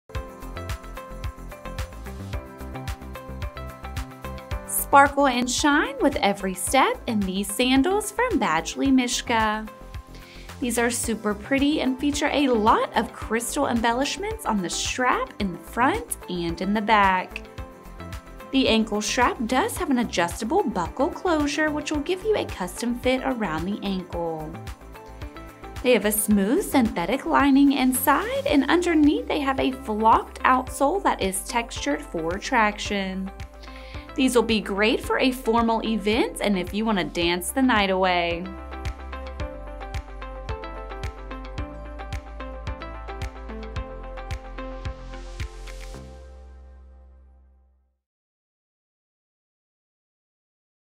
Badgley Mischka Kensey SKU: 9664467
Embellished with rhinestones and crystals on the straps, the  Badgley Mischka&reg; Kensey flat sandals offer a sparkly look.
Textile upper.
Synthetic lining and insole.
Ankle strap with buckle closure.
Open toe.
Flocking rubber outsole.
Imported.